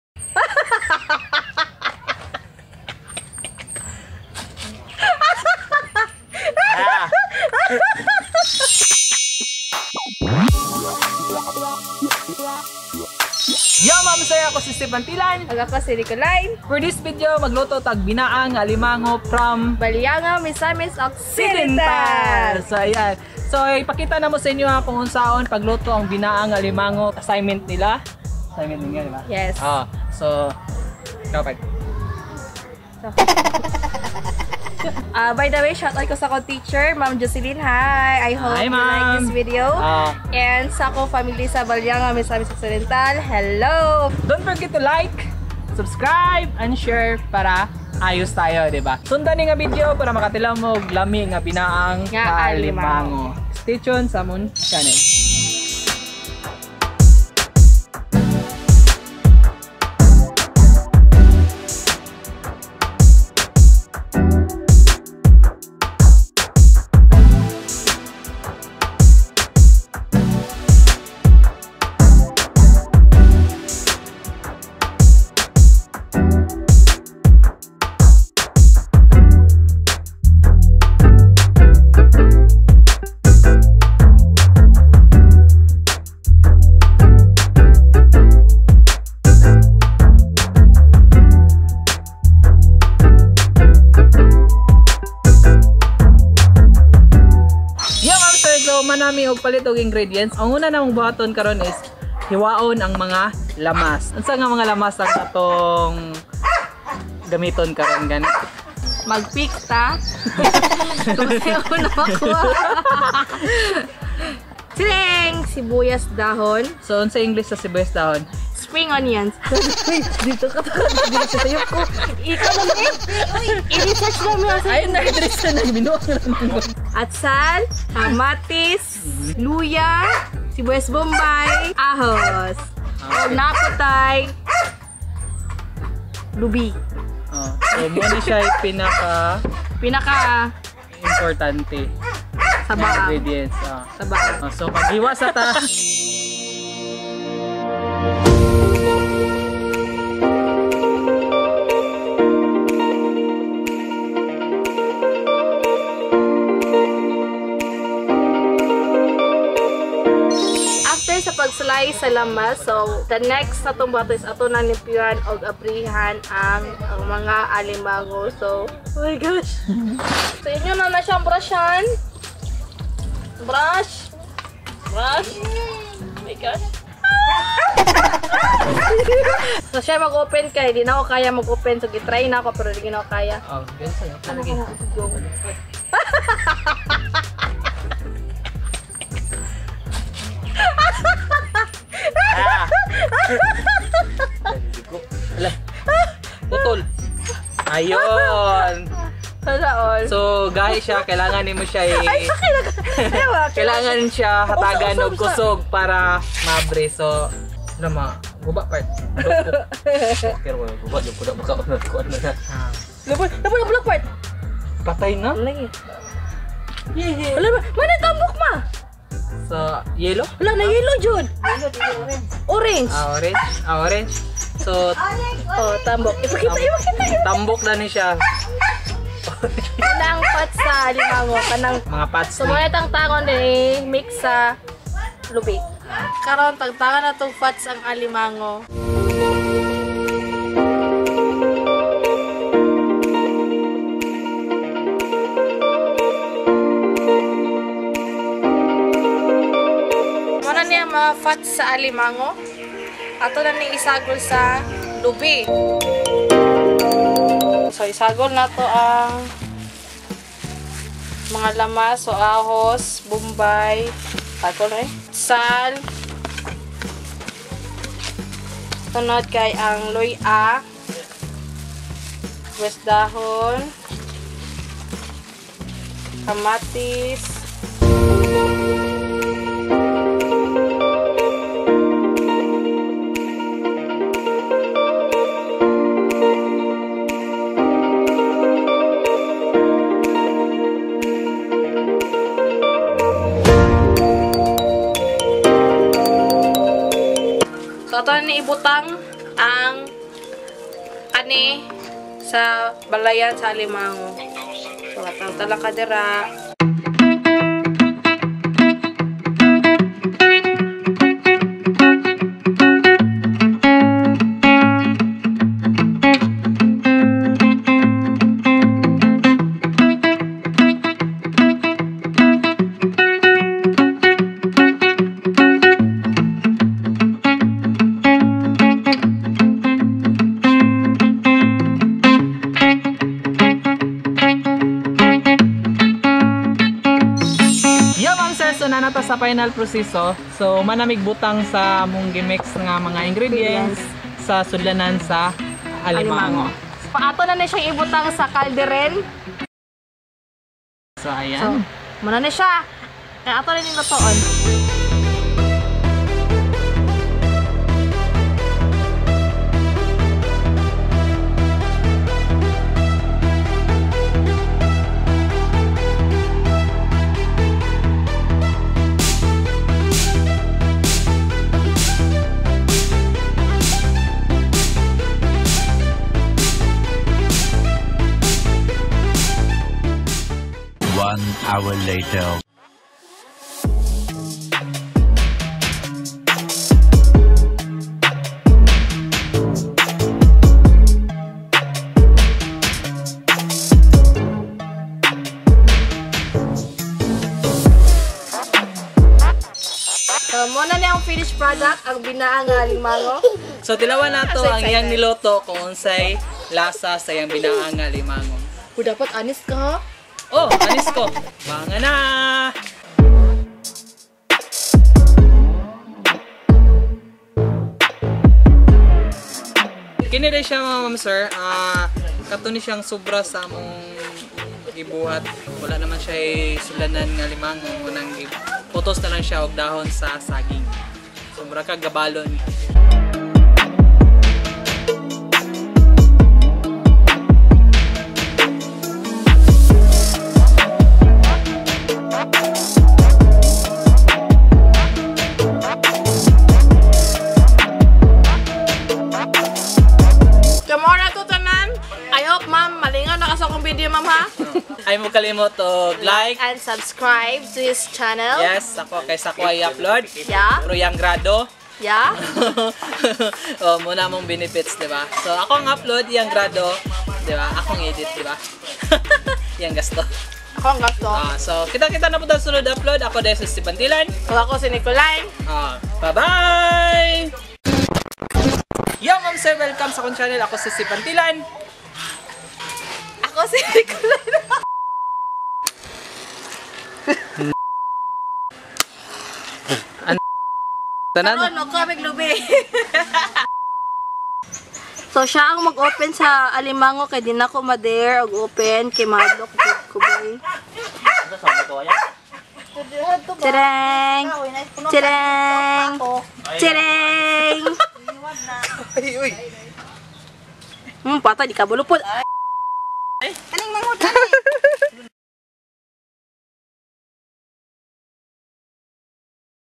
Binaang na Alimango 1st time magluto with my PARTNER
yow! mamser! welcome back sa aking channel for this video gagawa kami nang binaang na alimango o ginataang ALIMANGO para sa assignment ng aking partner para   sa kanilang summer class about sa kanilang family heirloom.. BINAANG NA ALIMANGO  naggikan pa sa iyang lolo og lola nga taga Baliangao, Misamis Occidental.. sana magustohan ninyo ni nga video...

DAGHANG SALAMAT